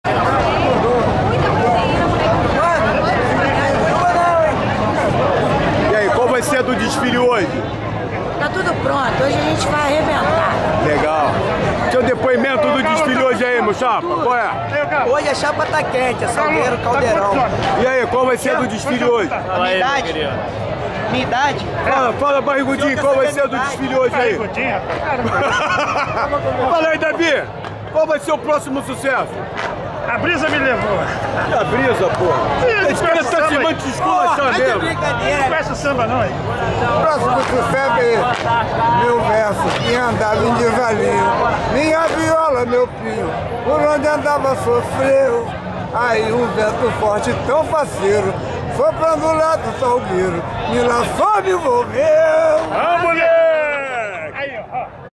Depoimento Compositores do Salgueiro - Ensaio Técnico 10/02/2012
Depoimento Compositores do Salgueiro - Ensaio Técnico 10/02/2012 - Direto da Marques de Sapucai/RJ - David Correia, Leonardo Galo, Maralhas, Kaka Salgueirense e Juarez.